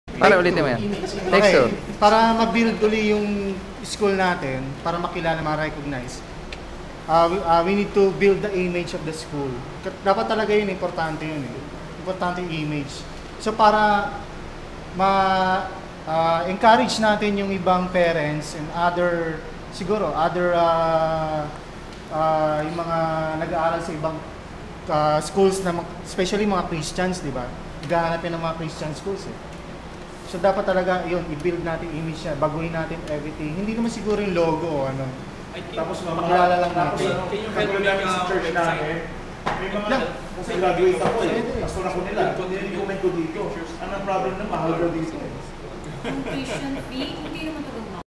Emerick for PR Man
Emerick volunteers himself to do the PR.  Go emerick.  Ikaw na gagawa ng lahat.  Nagbukas ka ng bibig mo.  Na kunana ko tuloy.  We have youtube as our witness.